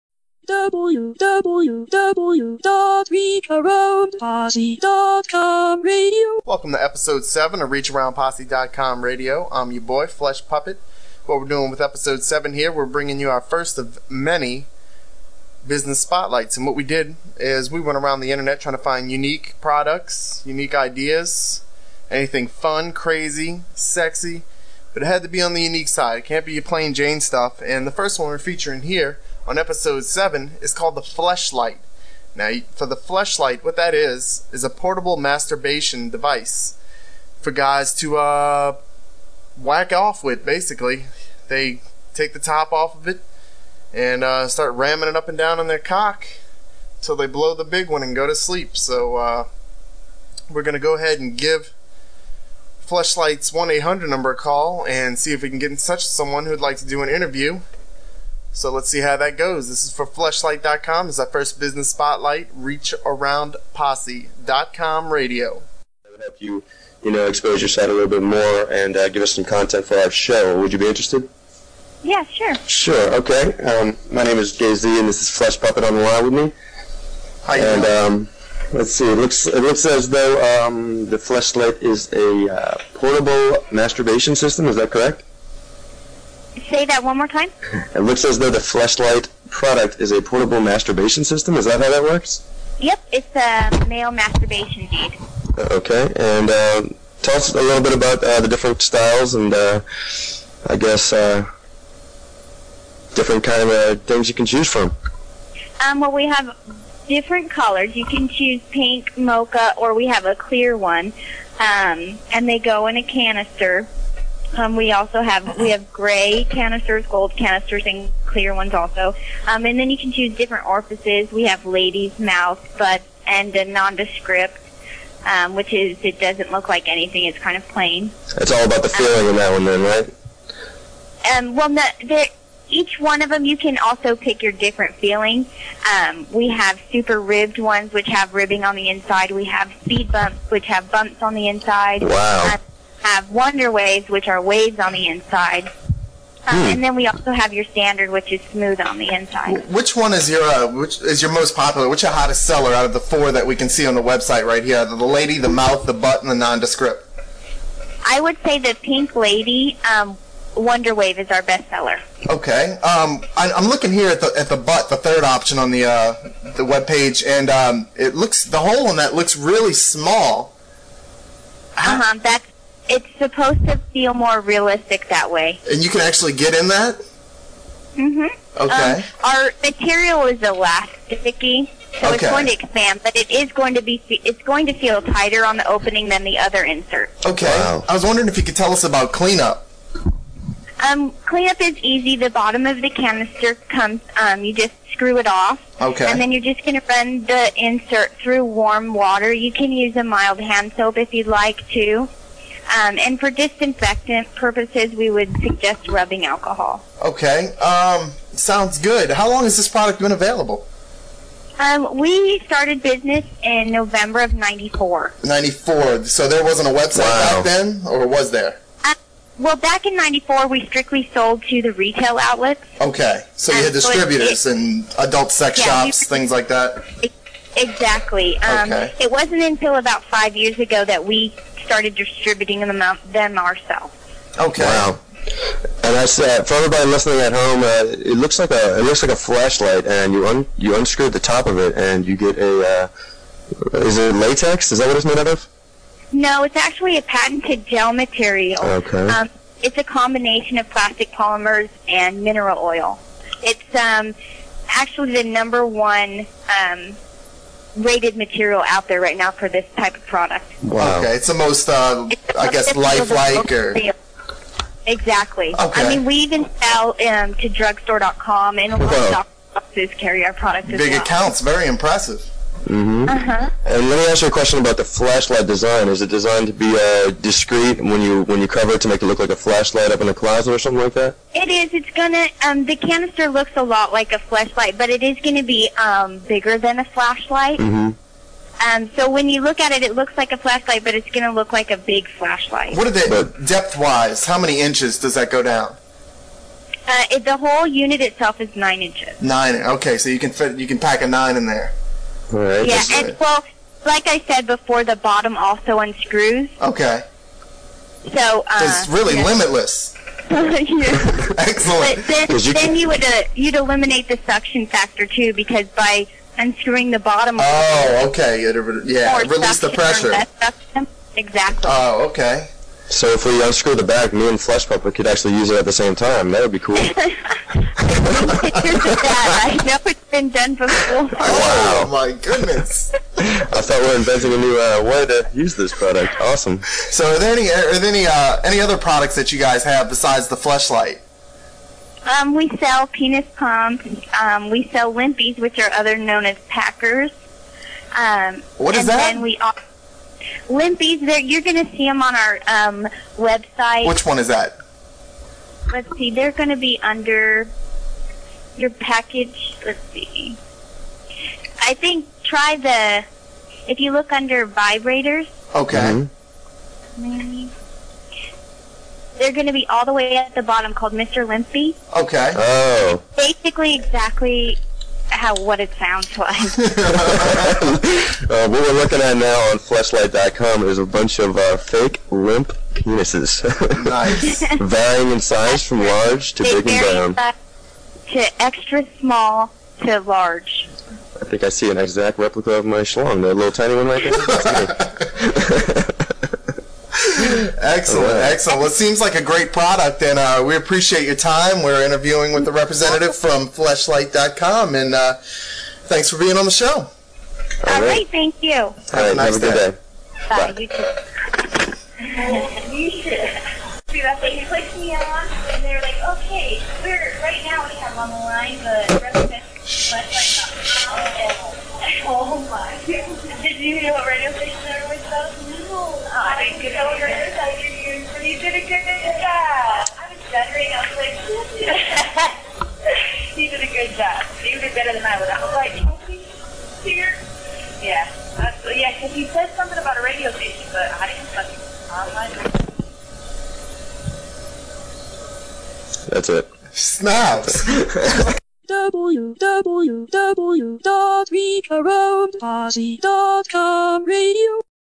2006 Fleshlight Phone Interview
Another interview from Jeff and I from way back in 2006. Fleshlight, back then, had just pushed their product. It was patented almost 10 years earlier, then blew up in the mid 2000's. We asked all the important questions.

I’ve made it easier than ever to help out.